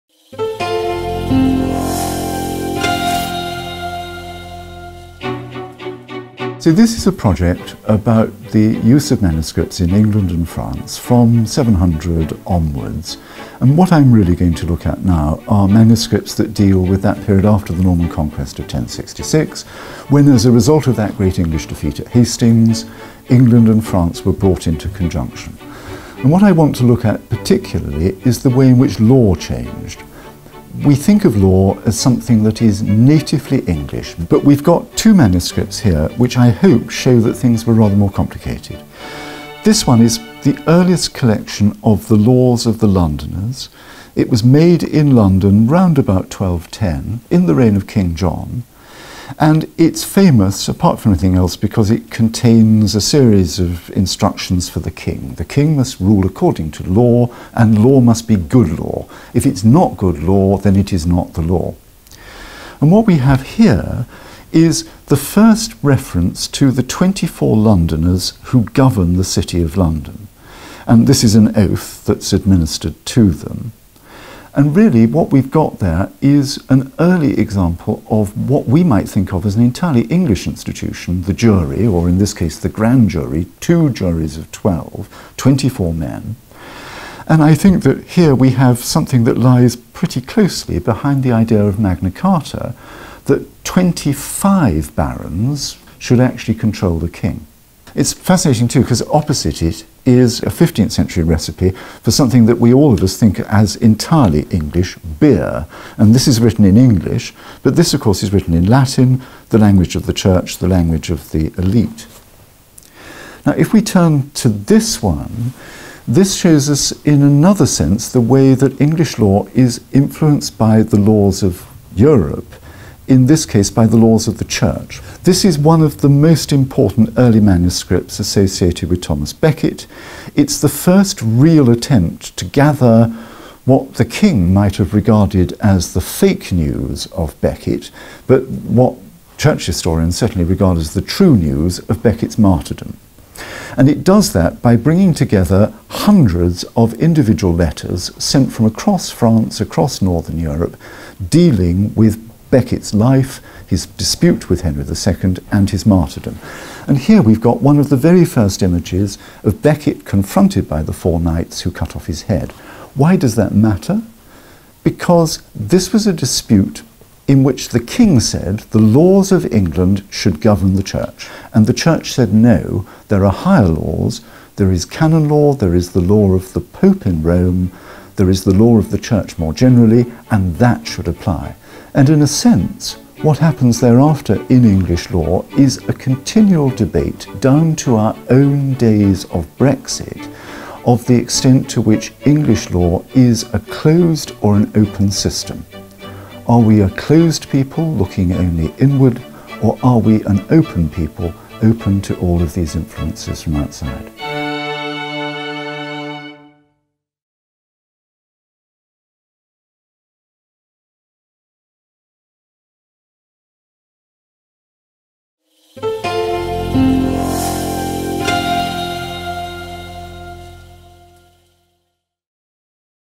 The Role of Law in Governing Medieval England
Nick Vincent discusses law-making and the relationship between the King and the Church in early medieval England.

Explore illuminated manuscripts from the national libraries of the UK and France